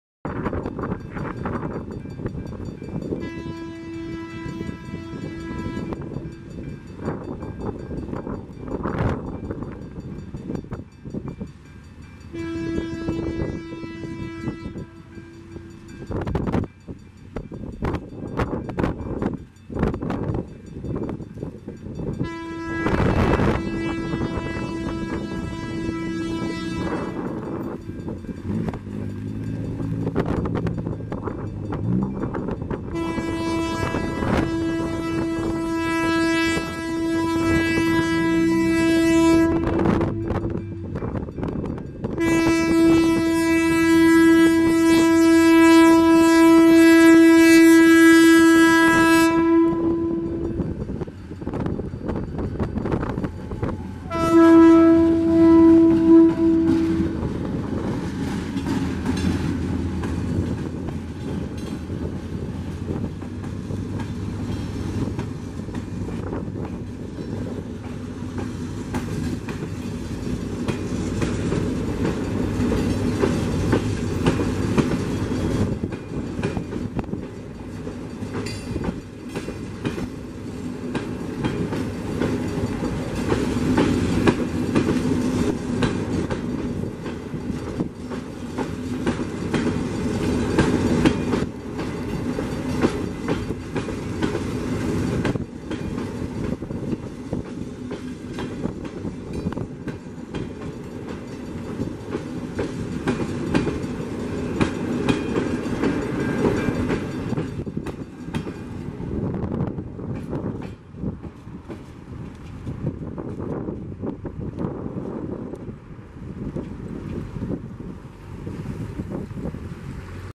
TREN DE PASAJEROS EL #268 BAJO LA LLUVIA PASANDO POR BºFERREYRA RUMBO A RETIRO BS.AS
locomotora : EMD-GT22CW Nº9026 + 10 COCHES CNR
fecha de filmacion : 23/10/2023
hora : 20:40
autor material : MARIANO TRENES
AGRADESCO a mi amigo MATIAS (conductor) y a su compañero por saludarme muy atentamente al pasar con esos lindos bocinasos y esa seña de luz.